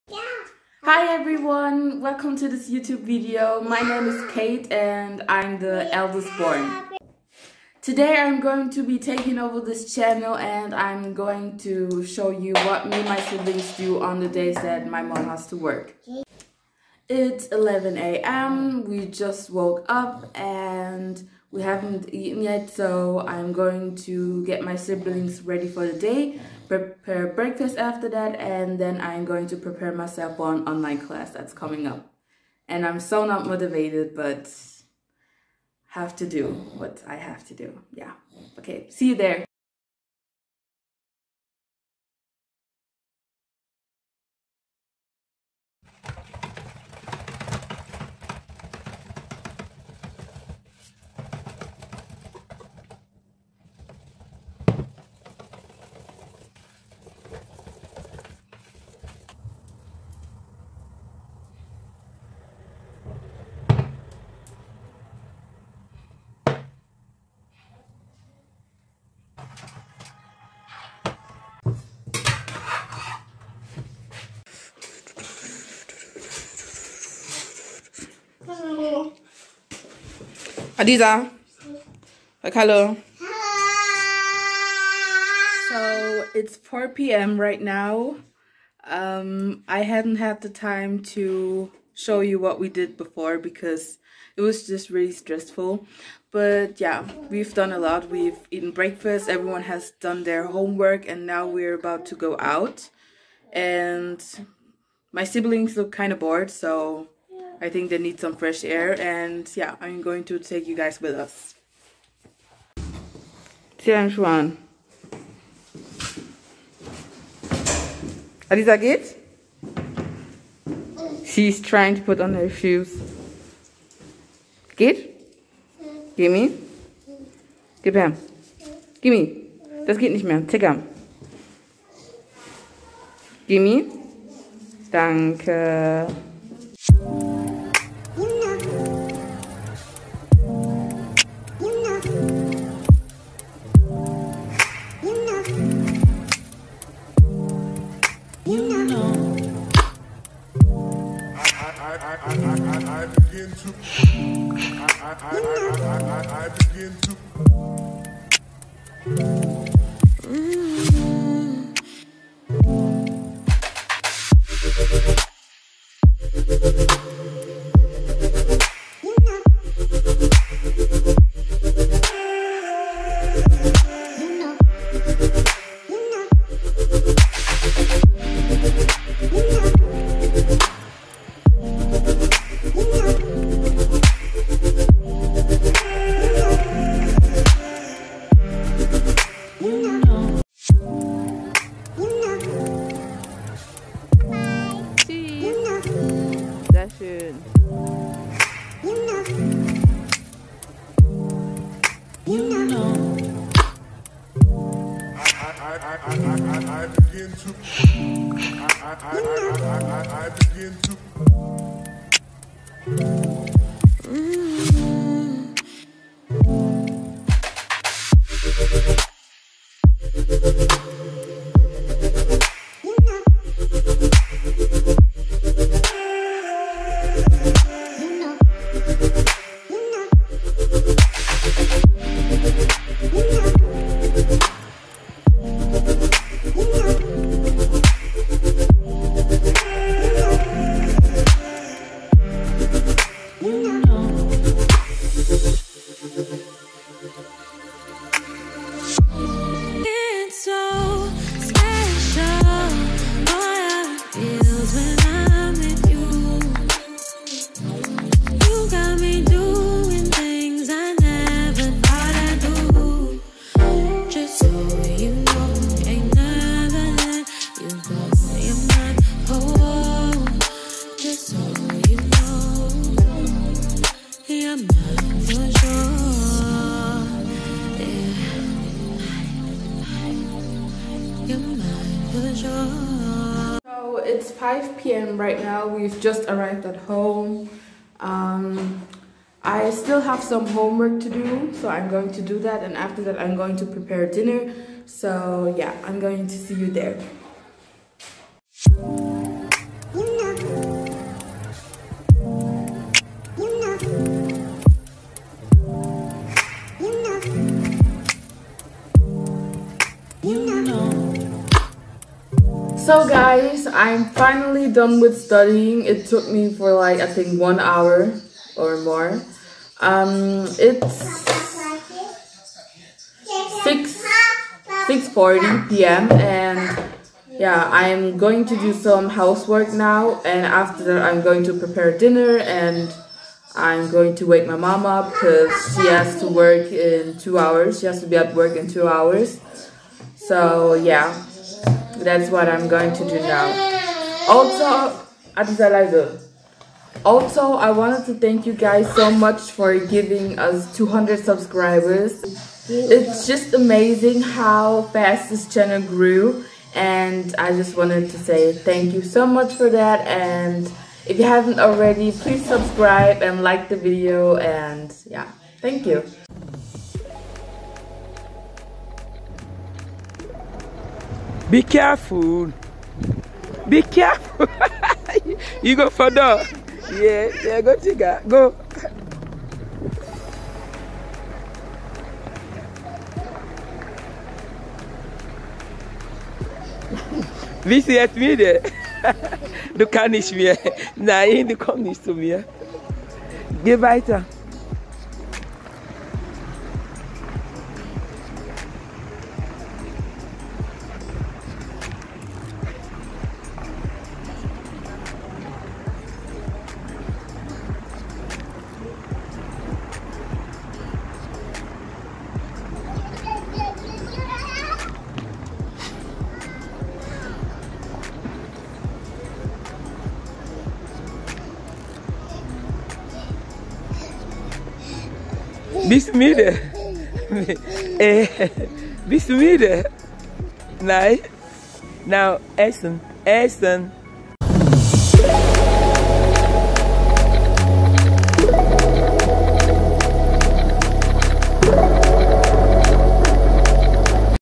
[Vlog 3 ]See what my elder daughter does wiht her siblings
Hi guys
My name is Cherie. I'm a single mother of 4 . Working as a RN full time.

In this video I going to show u what my kids  do on the days I work.

I know the edits  and English are not the best but together with u I will improve and better myself.

musican:  Jeff Kaale

Hope to see u again
Bye 💗